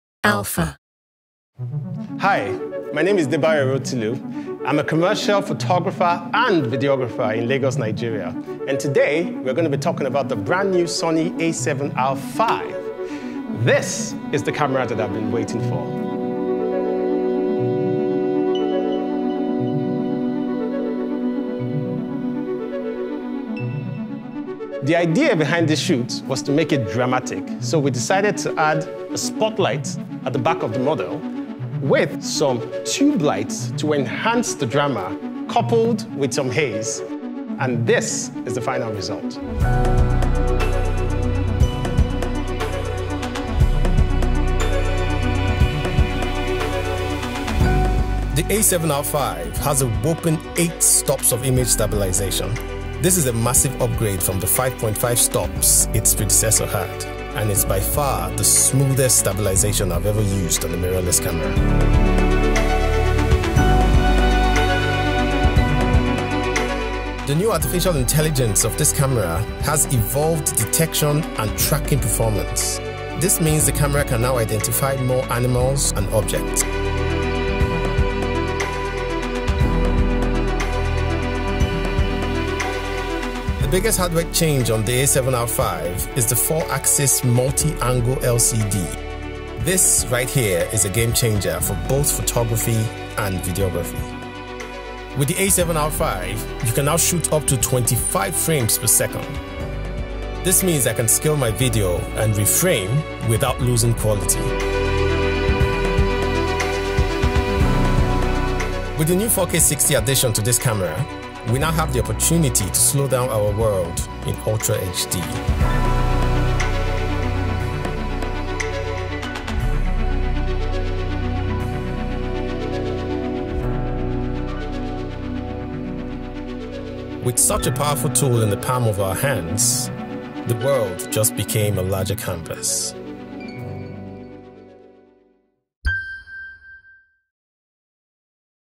"Reimagine" with Debayo Rotilu | Alpha 7RV | BTS
Reimagine.
Join Debayo Rotilu, a commercial photographer and videographer from Lagos, Nigeria, in this behind the scene experience as he leads you deeper into your imagination. Slow your world down and reimagine reality with the Sony Alpha 7R V.